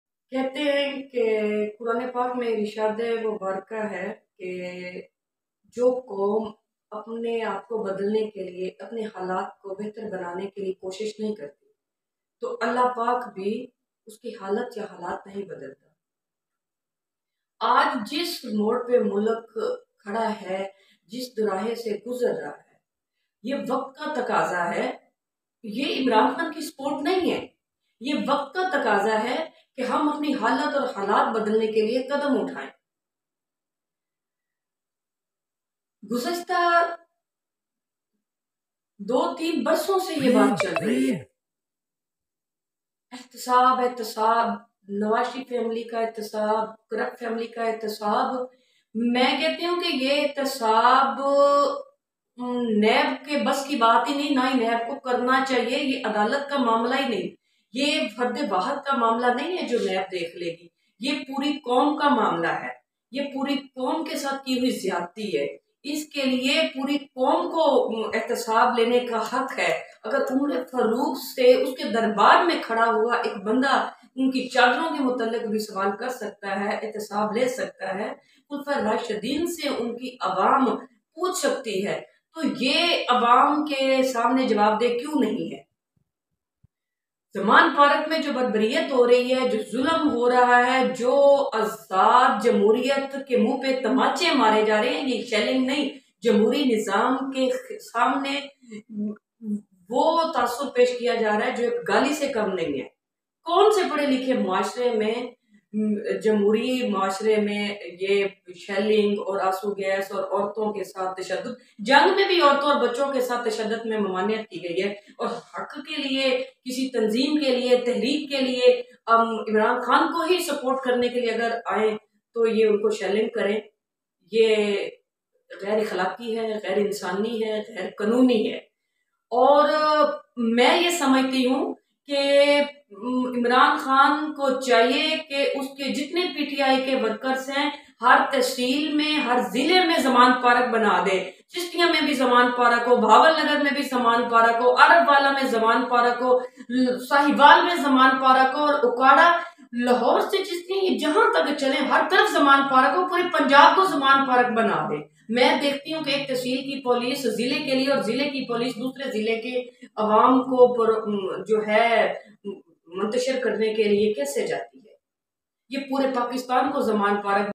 Miss Shahida Syed | Message To The Women Of Pakistan !
Miss Shahida Syed Is Famous Social Activist & Social Worker Form Tehsil Chishtian District Bahawalnagar.

In This Video She Talks About Importance Of Contribution Of Women In Political & Future Of Pakistan